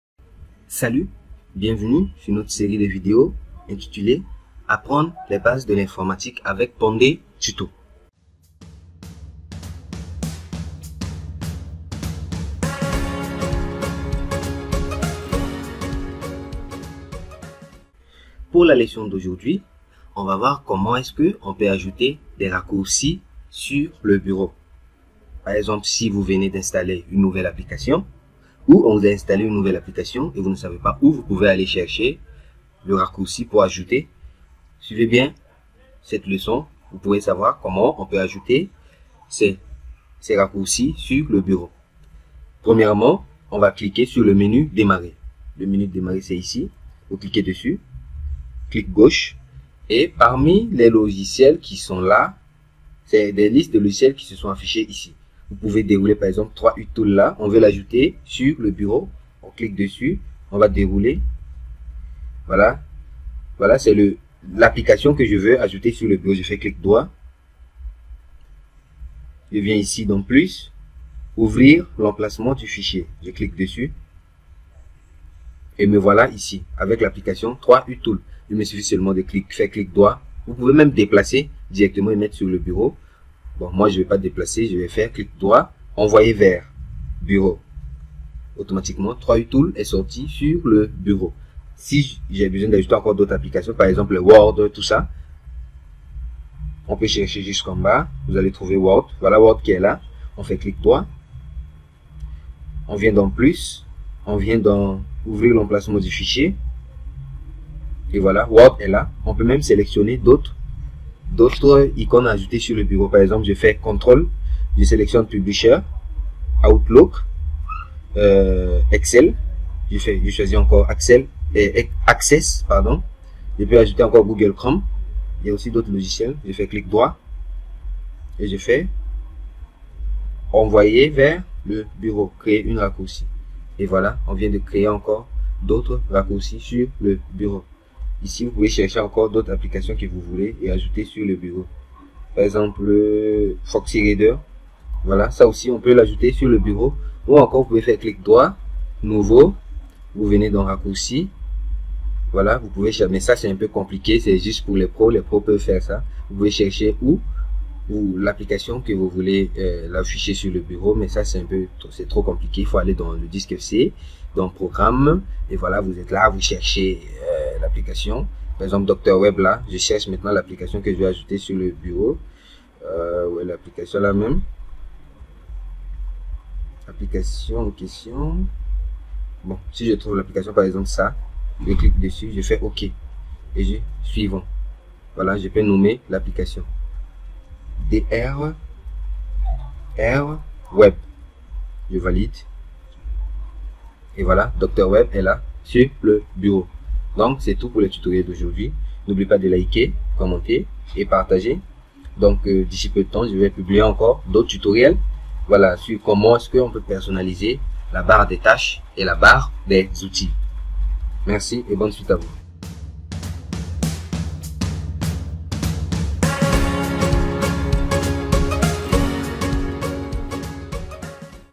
🛑 Comment créer des raccourcis sur le Bureau
Nous allons voir aujourd'hui comment ajouter des raccourcis sur le bureau Windows.

en suivant ce tutoriel vous saurez comment ajouter le raccourci de vos applications et dossier favoris sur le bureau pour y accéder en un temps record.